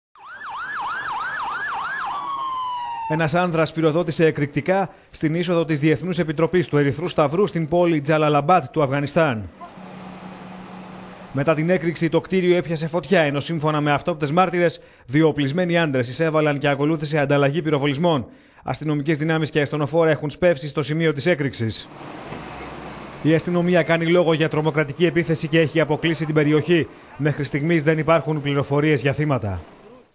Αφγανιστάν: Τρομοκρατική επίθεση κατά του Ερυθρού...
Ένας άνδρας πυροδότησε εκρηκτικά στην είσοδο της Διεθνούς Επιτροπής του Ερυθρού Σταυρού, στην πόλη Τζαλαλαμπάντ του Αφγανιστάν.

Μετά την έκρηξη, το κτίριο έπιασε φωτιά, ενώ σύμφωνα με αυτόπτες μάρτυρες δυο οπλισμένοι άντρες εισέβαλαν και ακολούθησε ανταλλαγή πυροβολισμών.

Αστυνομικές δυνάμεις και ασθενοφόρα έχουν σπεύσει στο σημείο της έκρηξης. Η αστυνομία κάνει λόγο για τρομοκρατική επίθεση και έχει αποκλείσει την περιοχή.

Μέχρι στιγμής, δεν υπάρχουν πληροφορίες για θύματα.

Είναι η δεύτερη επίθεση που σημειώνεται τις τελευταίες 24 ώρες. Τα ξημερώματα, έξι Ταλιμπάν σκοτώθηκαν, κατά τη διάρκεια επίθεσης εναντίον του γενικού επιτελείου του κυβερνήτη της επαρχίας Πανσίρ.